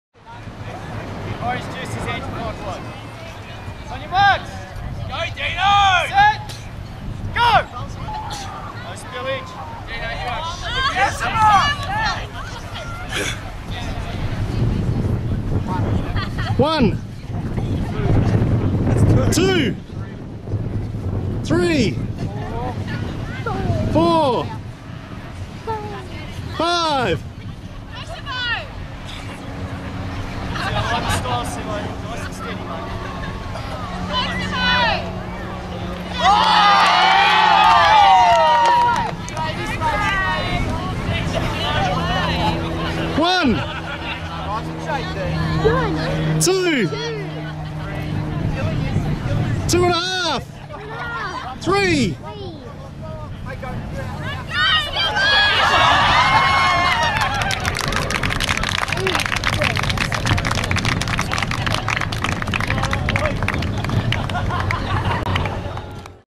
Around The Sticks - Dean v Simmo
City of Perth Surf Life Saving Club

End of Season - 28th March 2010

Around The Sticks Choc-Milk Challenge - Dean Scarff v Craig Simmonds